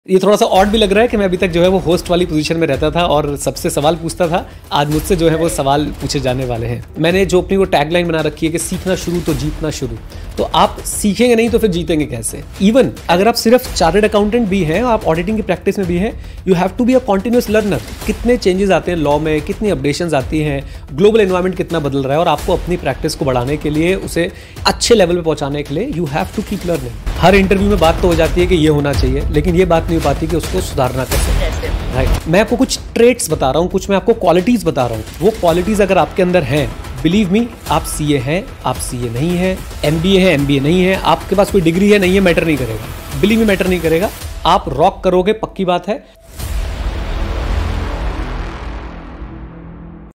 Teaser - Success Mantras by neerajarora sir
Watch the recently recorded full episode of Neeraj Arora Sir.

In this interview, we will get to know insights related to what skills one must possess to succeed in life, how to enter into venture capital, the role of networking, and how Chat GPT and AI can support grooming a CA in this technologically advanced era.
Enjoy the podcast. Keep learning, Keep growing Kyoki Seekhna Shuru to Jeetna Shuru👍